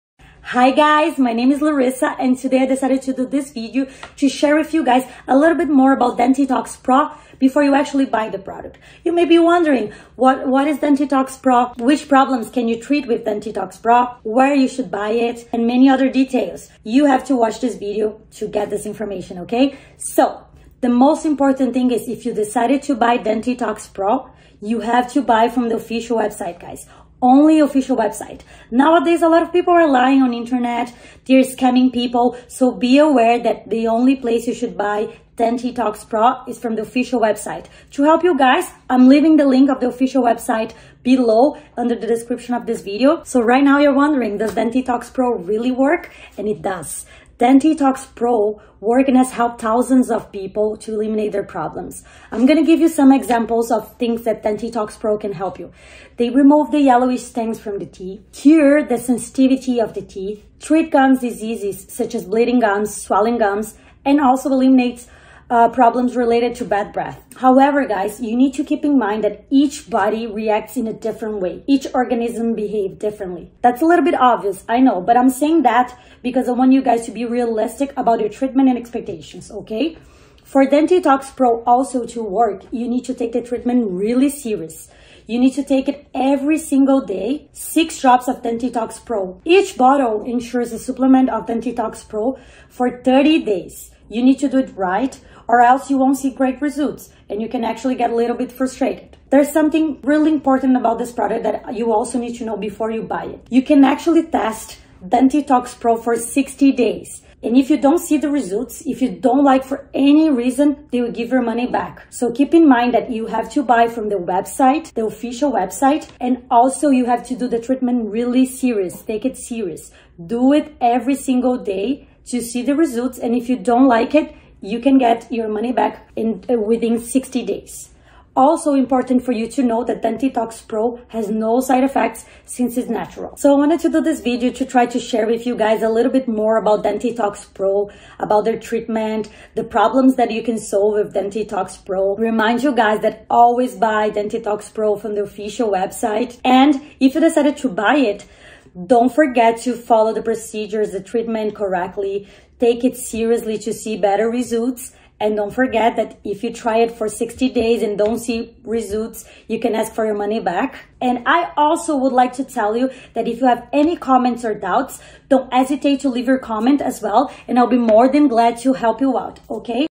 DENTITOX PRO REVIEW – DENTITOX PRO – Dentitox Pro Mouth Review -Watch This ⚠ - Dentitox Pro Reviews
Today I’ll tell you everything you need to know about Dentitox Pro Review before you actually buy the product. I also have two very important alerts, so pay close attention to what I have to say.

What is Dentitox Pro?
Dentitox Pro is an oral health supplement. This tooth drop is intended to restore your gums and teeth. Above all, claims to help in the prevention of dental caries. The inspiration for the Dentitox Pro Review came from a secret discovered in the ancient rituals of African tribes.
Dentitox is formulated with all-natural components to promote good oral health, strong teeth and fresh breath. Professor Marc Hall is the creator of Dentitox Pro. He came up with this mixture while looking for a way to deal with his bad dental health. He and his doctor friend discovered this substance as a remedy for their different teeth and oral health problems. After being able to solve his own oral problem, the creator decided that he should help more people, and so Dentitox appeared.

How Dentitox Pro Works
The Dentitox Pro formula focuses on delivering nutrients that the body needs. Within just 6 drops of the formula, consumers will get a ton of different nutrients that their body already needs to maintain for better health, like vitamins A, C, D3, and K2. It even includes many minerals like potassium and calcium.
However, with all of these typical ingredients, consumers would seemingly get the same support from simply using a multivitamin. That’s why there are additional ingredients that make the performance of this remedy even more intense, including:
• Elderberry
• Ironized phosphoric acid
• Xylitol
• Collagen
• MSM
• Indian licorice root extract
• Neem
• Sage
• Cinnamon essential oil

Whenever the user decides to include the drops of Dentitox Pro in his routine, he must apply them directly to the teeth and gums. Brushing your teeth is still necessary to maintain oral health, but Dentitox Pro provides a way to increase that health.
There are no side effects. Dentitox Pro drops is a 100% natural supplement without chemicals, harmful substances to bring any side effects with regular use. To make it clear, the Dentitox Pro supplement is safe for regular use.
There is something really important about this product that you need to know before you buy it: you can actually test Dentitox Pro review for 60 days and if you don’t see the results, if you don’t like it for any reason, they will give you your money back. Also, for Dentitox Pro to work, you need to take the treatment seriously.